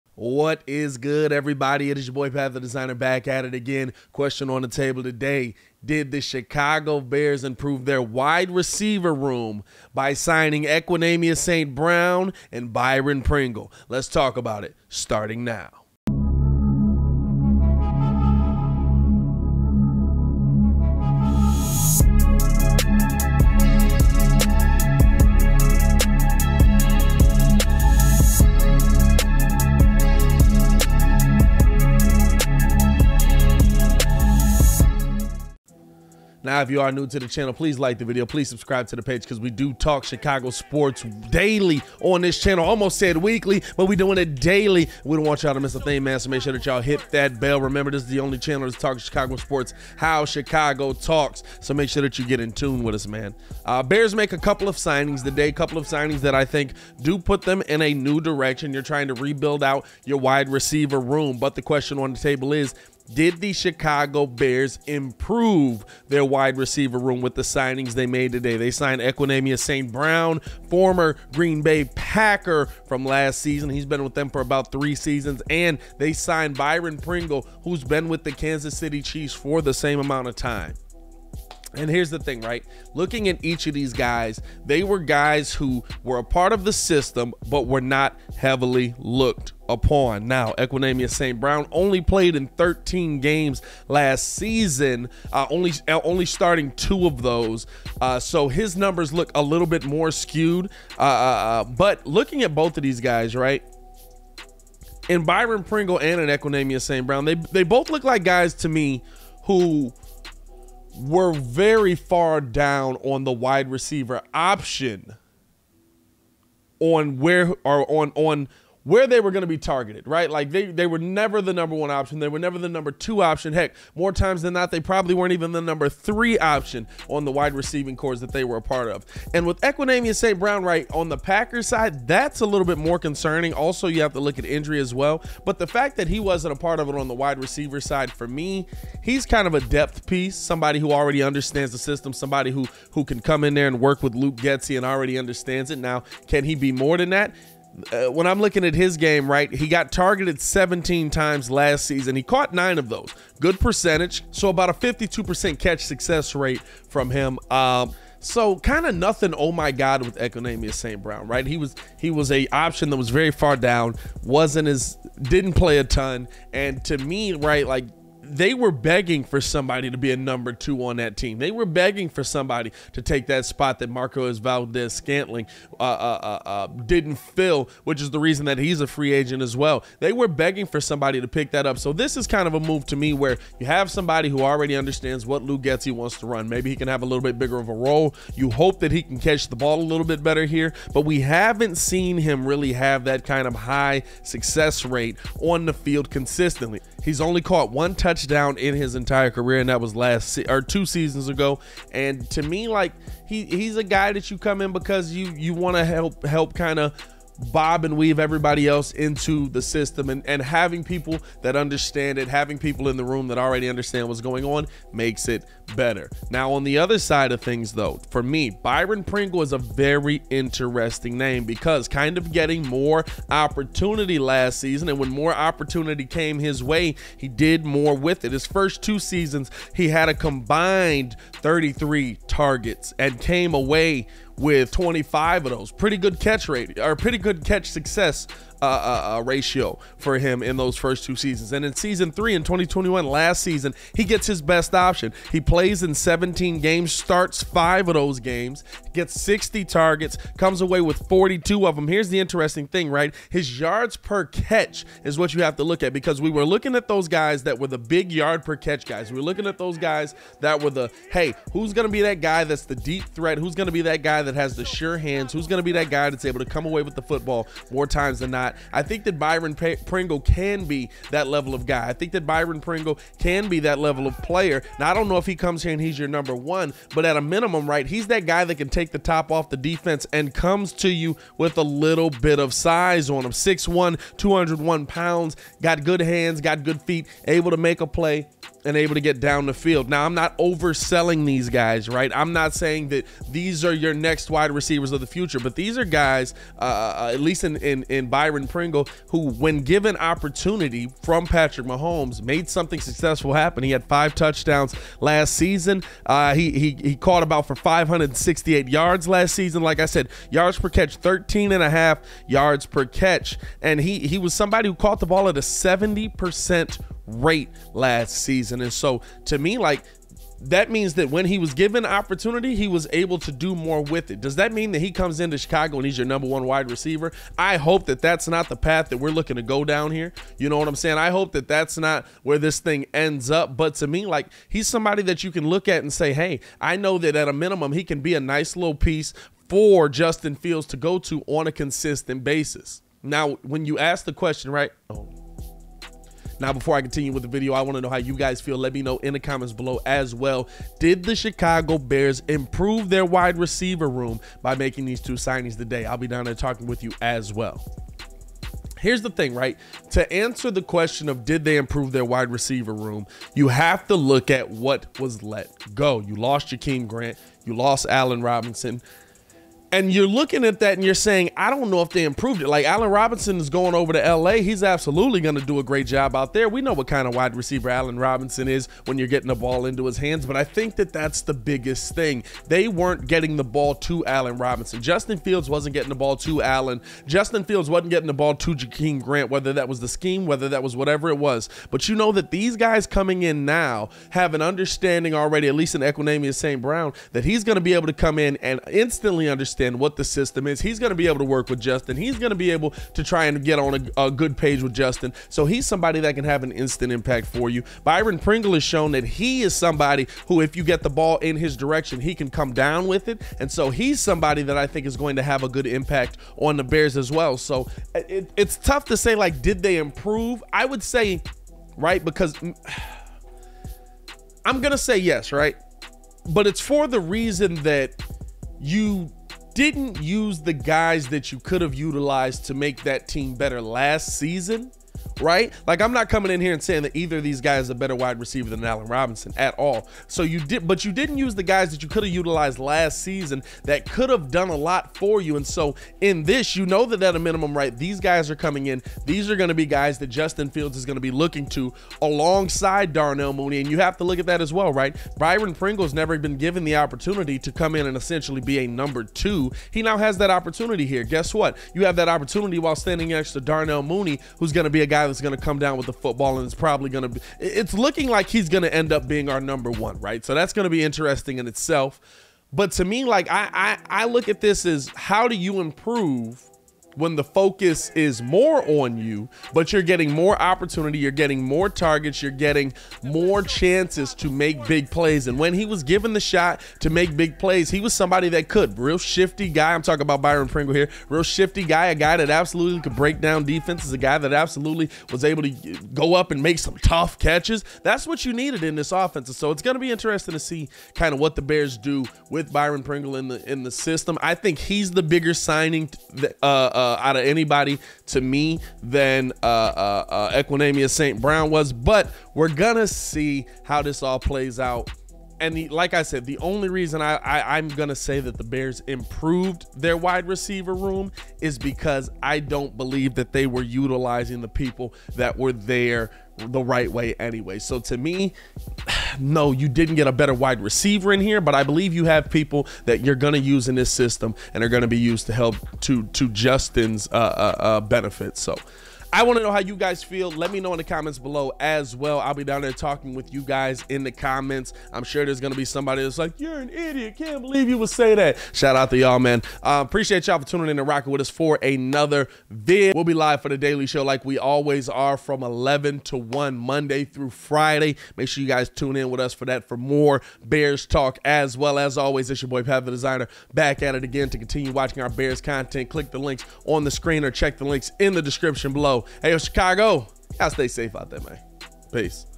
Did The Chicago Bears Improve Their Receiver Room Yesterday ?
Pat The Designer Breaks down the signings of Equanimeous St. Brown & Byron Pringle & Asks If The Chicago Bears Improve Their Receiver Room Yesterday ?

Did The Bears Improve Yesterday?

BECOME A BREEZE SUPER FAN !